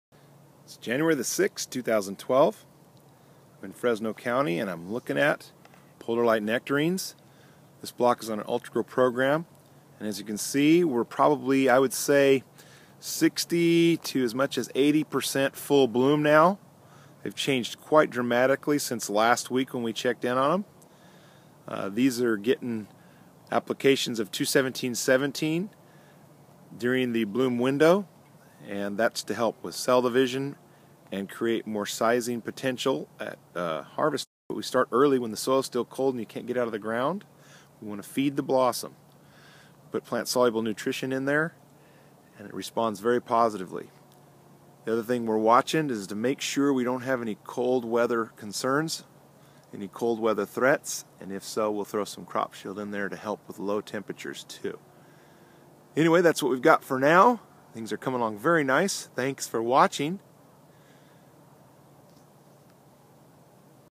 Marc Suderman #82 - Polar Light nectarine: 2/6/12
What a difference a week makes! The bloom looks great!

Here in Fresno county California, we have been experiencing warmer than usual temperatures this year. For the past two weeks, daytime temps have been in the 60's. This has really kept the bloom moving rapidly.

This field is on a customized fertility protocol.

Bloom stage is a critical stage of plant development. We have found that (depending on temperature) cell-division stage lasts 23 to 27 days after fertilization happens.
When bloom occurs, soil temperature is too cold to release P (below 63-68F) from the soil into the tree roots.

Phosphate at this stage is important because it stimulates cell division. More cell division = More size at harvest.

The important key to this being successful is the solubility of the phosphate. Orthophosphate is the form of phosphate that a plant takes up.